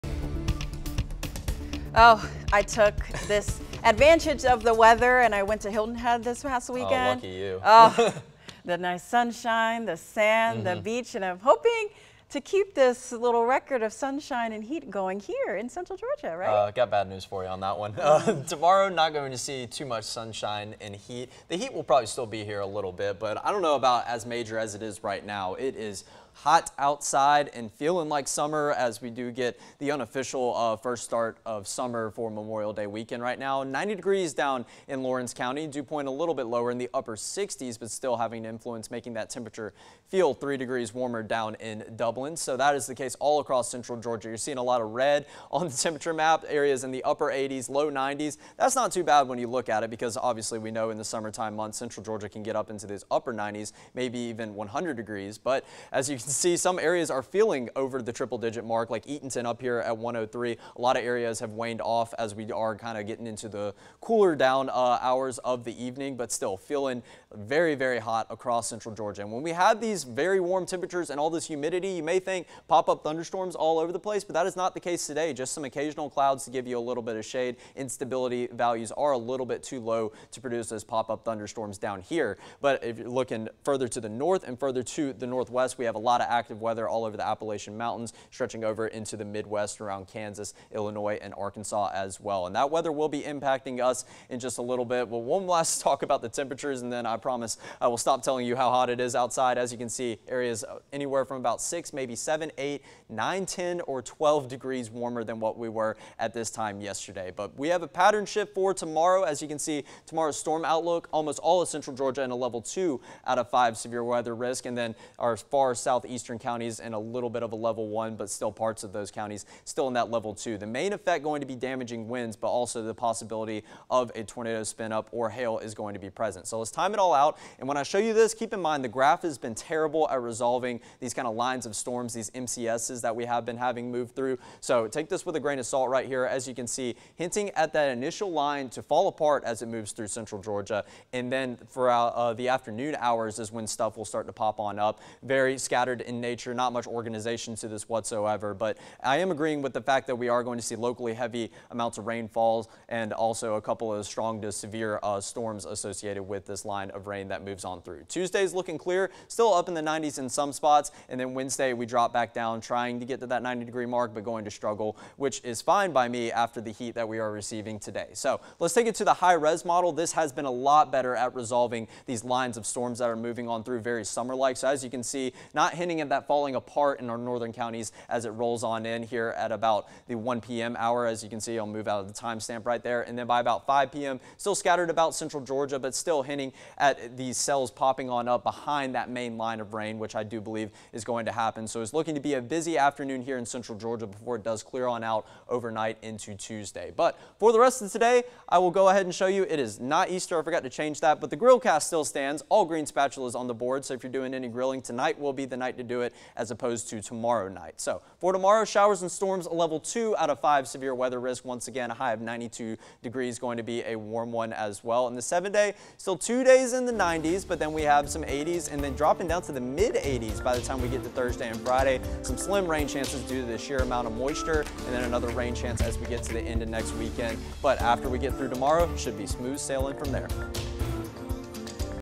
Alex's Sunday Evening Forecast
Showers and storms for Memorial Day afternoon